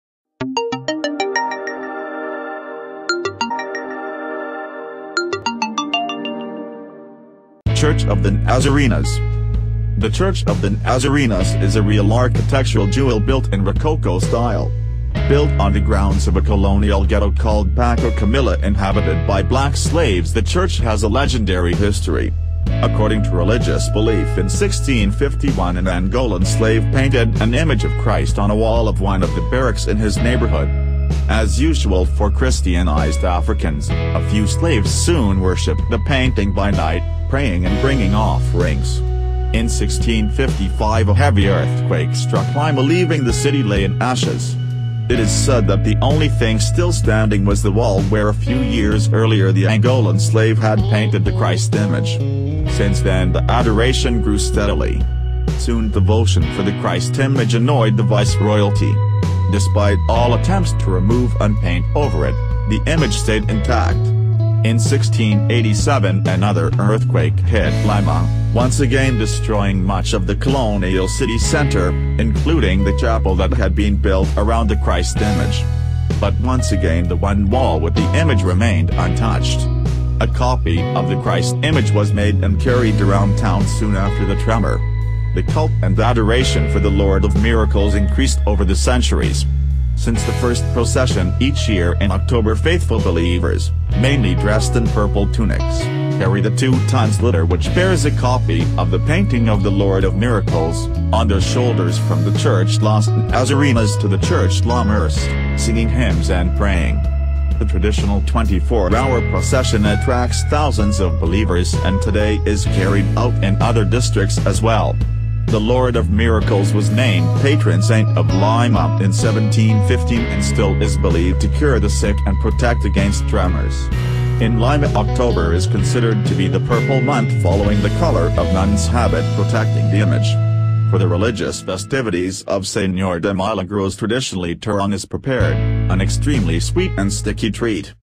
Church of the Nazarenas, Lima / Tourist Destination Peru
According to religious belief in 1651 an Angolan slave painted an image of Christ on a wall of one of the barracks in his neighborhood. As usual for Christianized Africans, a few slaves soon worshipped the painting by night, praying and bringing offerings. In 1655 a heavy earthquake struck Lima leaving the city lay in ashes. It is said that the only thing still standing was the wall where a few years earlier the Angolan slave had painted the Christ image. Since then the adoration grew steadily. Soon devotion for the Christ image annoyed the Viceroyalty. Despite all attempts to remove and paint over it, the image stayed intact. In 1687 another earthquake hit Lima, once again destroying much of the colonial city center, including the chapel that had been built around the Christ image. But once again the one wall with the image remained untouched. A copy of the Christ image was made and carried around town soon after the tremor. The cult and adoration for the Lord of Miracles increased over the centuries.